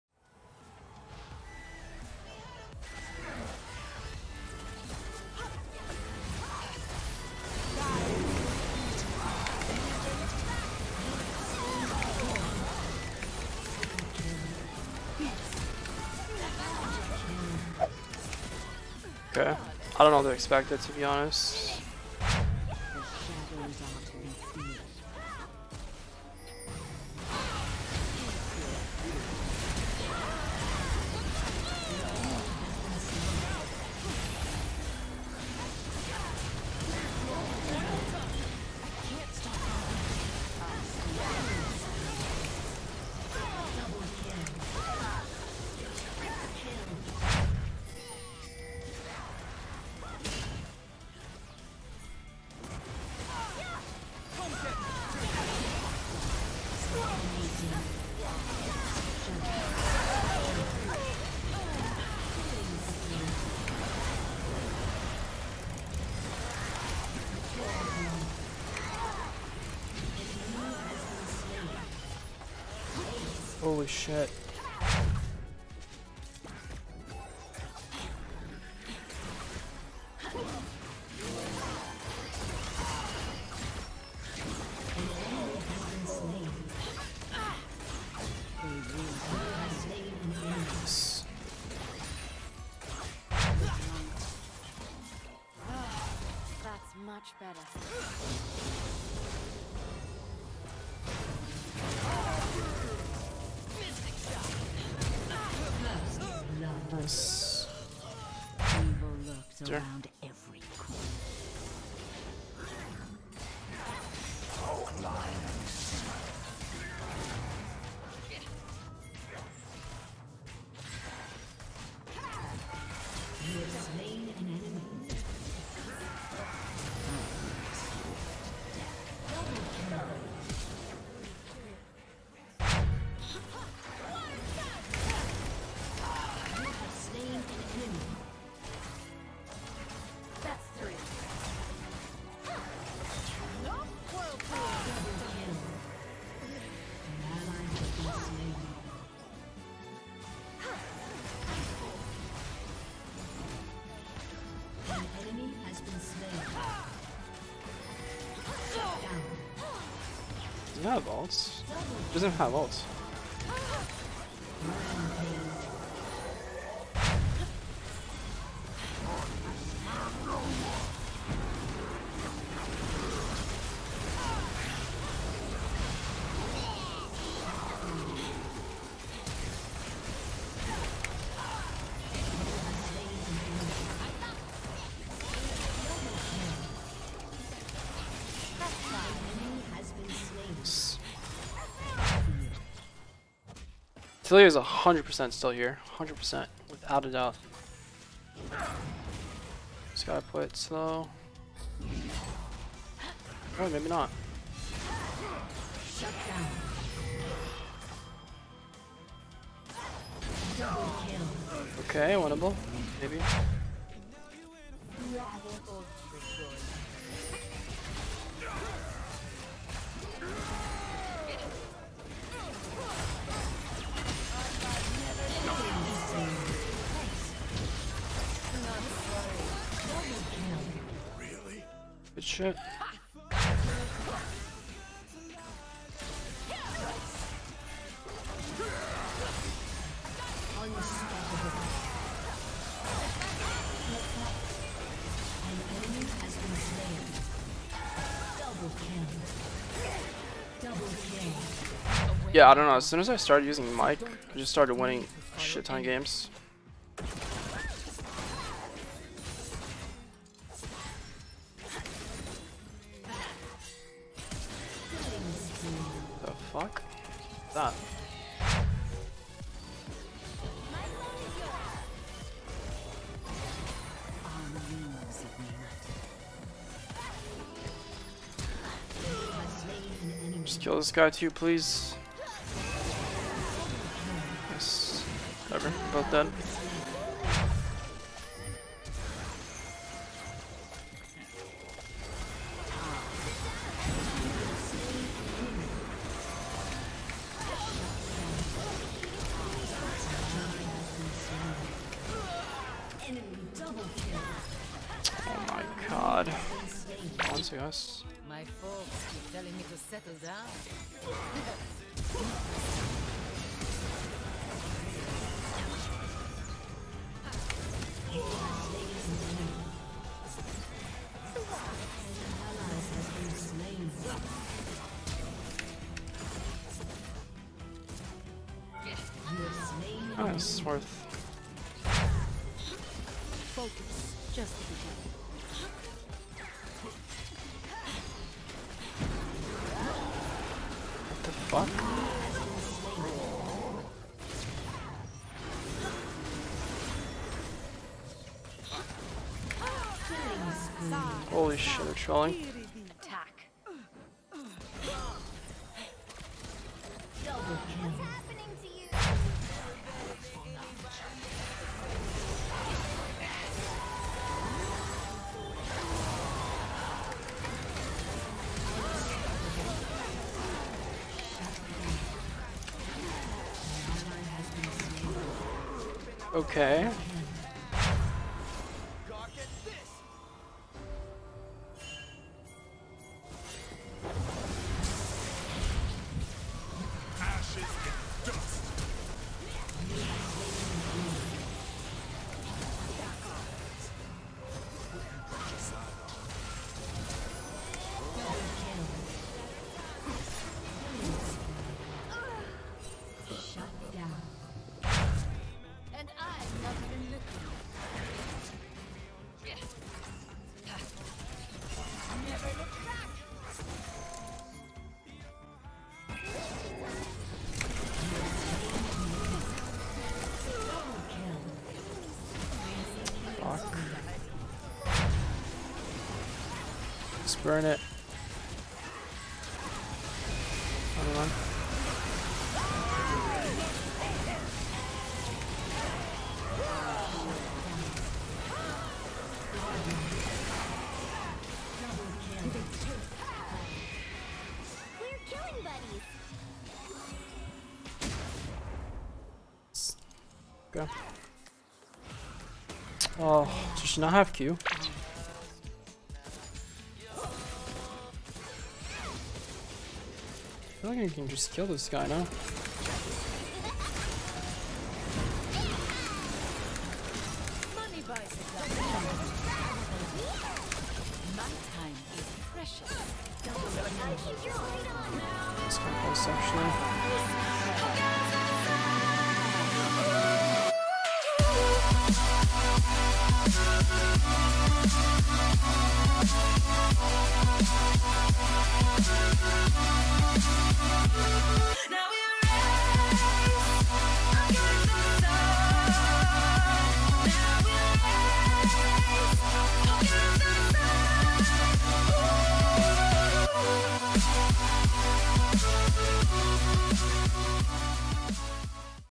Gosu - 431
Leave a "Like" if you enjoyed the video! :D

This video is a compilation of stream highlights, containing high elo gameplay.